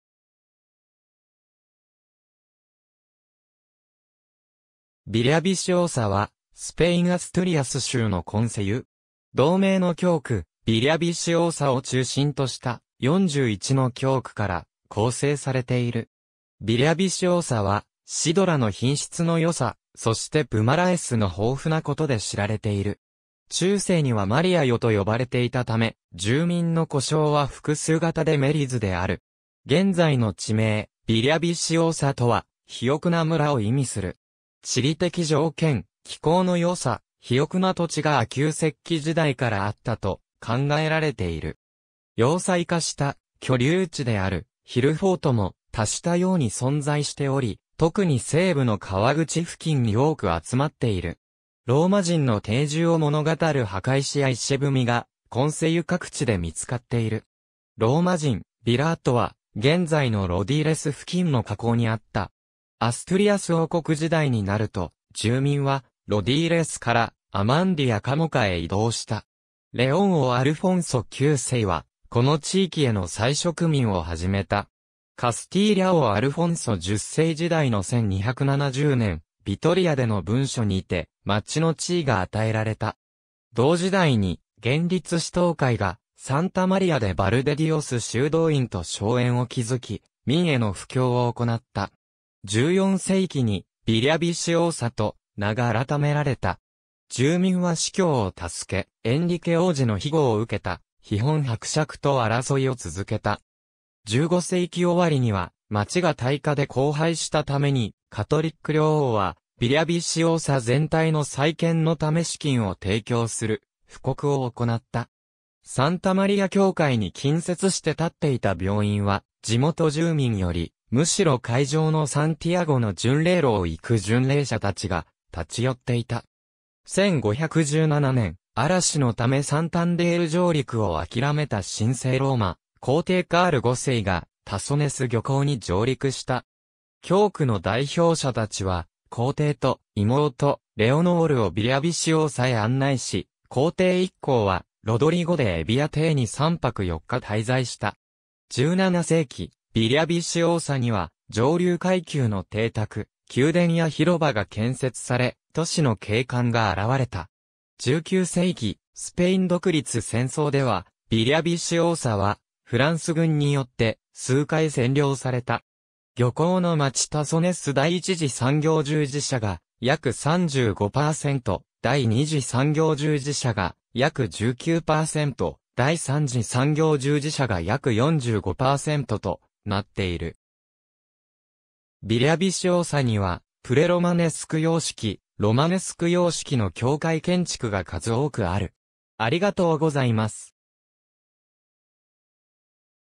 ビリャビシオサ
ビリャビシオーサ（スペイン語: Villaviciosa）は、スペイン・アストゥリアス州のコンセユ（基礎自治体）。
同名の教区ビリャビシオーサを中心とした、41の教区から構成されている。
ビリャビシオーサは、シドラ（リンゴ種）の品質の良さ、そしてプマラエス（Pumaraes、アストゥリアスでリンゴの果樹園を意味する）の豊富なことで知られている。
中世にはマリアヨ(Maliayo)と呼ばれていたため、住民の呼称は複数形でmaliayésである。
現在の地名ビリャビシオーサとは、「肥沃な村」を意味する。
地理的条件、気候の良さ、肥沃な土地が亜旧石器時代からあったと考えられている。
要塞化した居留地であるヒルフォート（スペイン語でCastro）も多種多様に存在しており、特に西部の河口付近に多く集まっている。
ローマ人の定住を物語る墓石や碑がコンセユ各地で見つかっている。
ローマ人ヴィラ跡は、現在のロディーレス付近の河口にあった。
アストゥリアス王国時代になると、住民はロディーレスからアマンディやカモカへ移動した。
レオン王アルフォンソ9世は、この地域への再植民を始めた。
カスティーリャ王アルフォンソ10世時代の1270年、ビトリアでの文書にて町（または村）の地位が与えられた。
同時代に、厳律シトー会がサンタ・マリア・デ・バルデディオス修道院と荘園を築き、民への布教を行った。
14世紀にビリャビシオーサと名が改められた。
住民は司教を助け、エンリケ王子の庇護を受けたヒホン伯爵と争いを続けた。
15世紀終わりには町が大火で荒廃したために、カトリック両王はビリャビシオーサ全体の再建のため資金を提供する布告を行った。
サンタ・マリア教会に近接して建っていた病院は、地元住民よりむしろ海上のサンティアゴの巡礼路を行く巡礼者たちが立ち寄っていた。
1517年、嵐のためサンタンデール上陸をあきらめた神聖ローマ皇帝カール5世が、タソネス漁港に上陸した。
教区の代表者たちは、皇帝と妹レオノールをビリャビシオーサへ案内し、皇帝一行はロドリーゴ・デ・エビア邸に3泊4日滞在した。
17世紀、ビリャビシオーサには上流階級の邸宅、宮殿や広場が建設され、都市の景観が現れた。
19世紀、スペイン独立戦争ではビリャビシオーサはフランス軍によって数回占領された。
漁港の町タソネス 第一次産業従事者（牧畜、酪農、漁業）が約35％、第二次産業従事者（飲料、特にシドラ）が約19％、第三次産業従事者（特に観光業）が約45％となっている。
ビリャビシオーサには、プレロマネスク様式、ロマネスク様式の教会建築が数多くある。